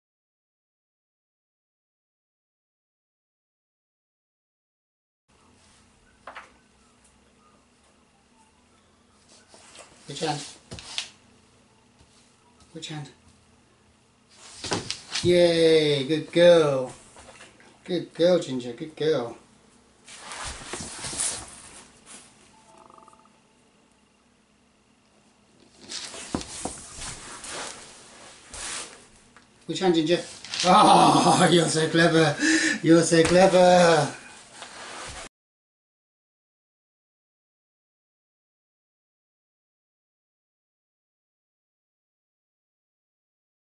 Gingers Boredom Buster - Tip5
Why not teach your dog to play the 'Guess the hand game'. This is great for build that strong relationship with your dog. And it really makes your dog think. You can see the concentration on Gingers face. Don't let your dog see what hand you put it in to start with, they'll always try and cheat!